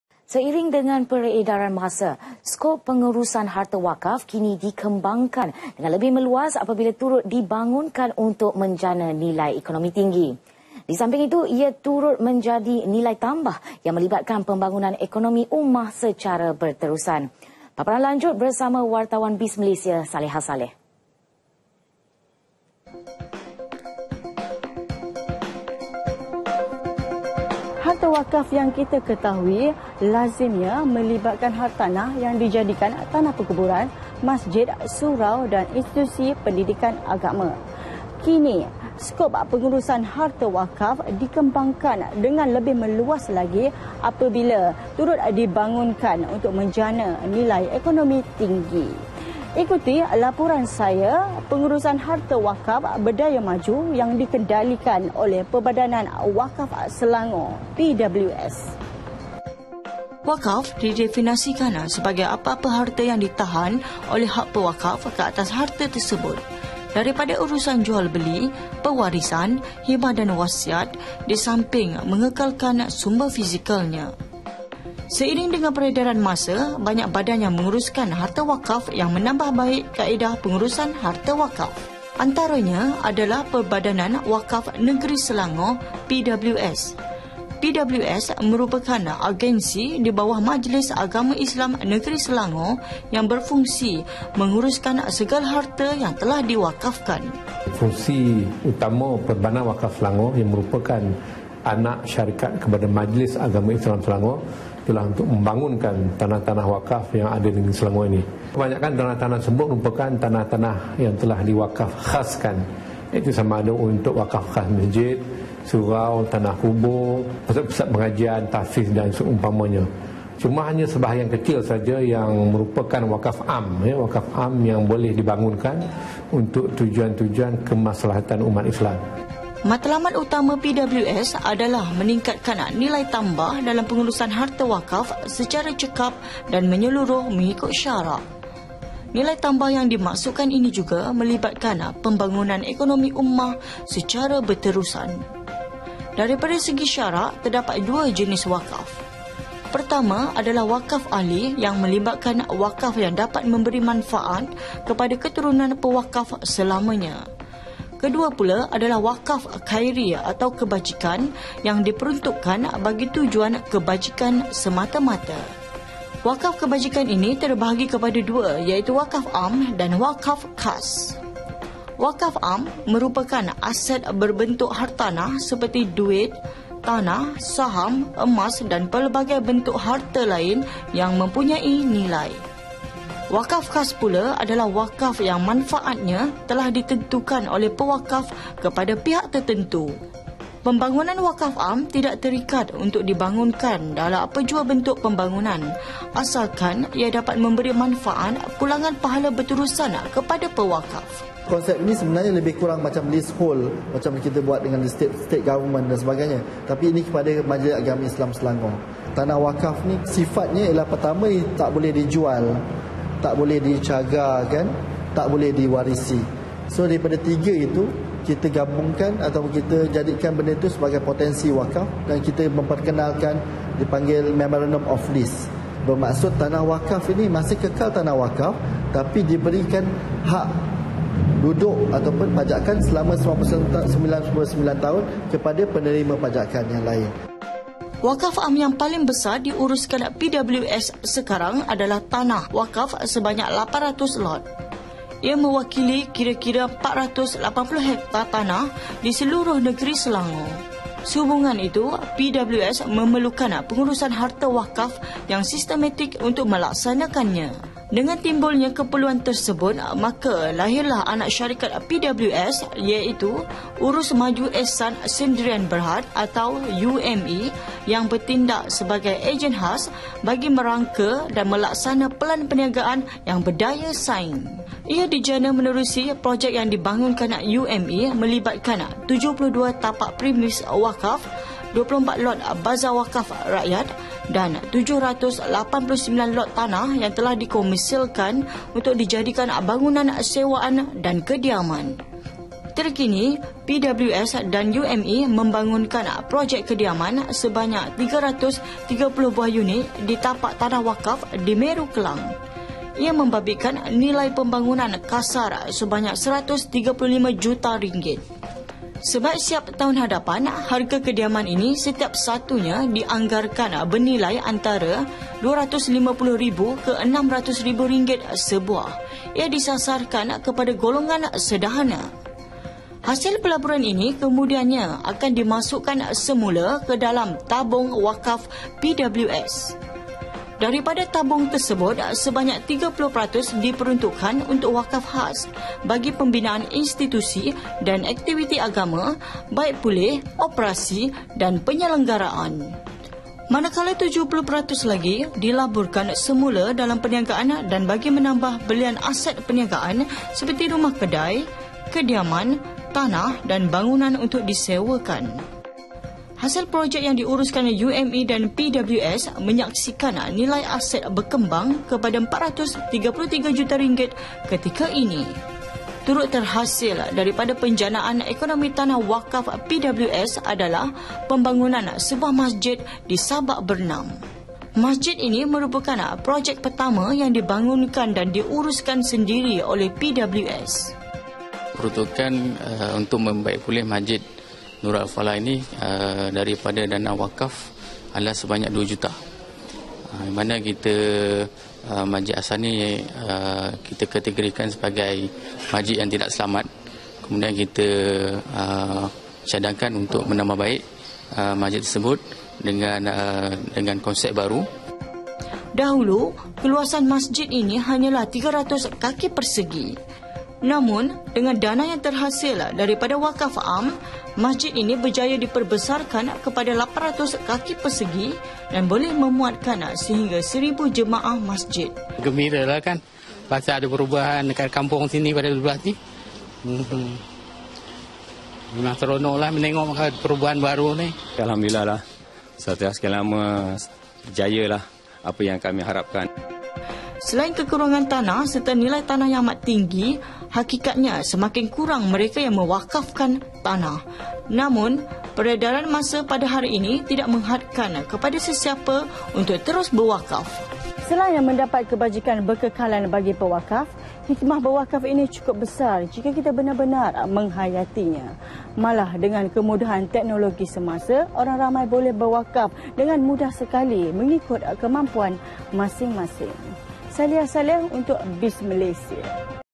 BIZ MALAYSIA - SKOP PENGURUSAN HARTA WAKAF [ 18 APRIL 2016]
Seiring dengan peredaran masa, skop pengurusan harta wakaf kini dikembangkan dengan lebih meluas apabila turut dibangunkan untuk menjana nilai ekonomi tinggi.

Disamping itu, ia juga menjadi nilai tambah yang melibatkan pembangunan ekonomi ummah secara berterusan.

Paparan lanjut bersama Wartawan BizMalaysia, SALIHA SALLEH.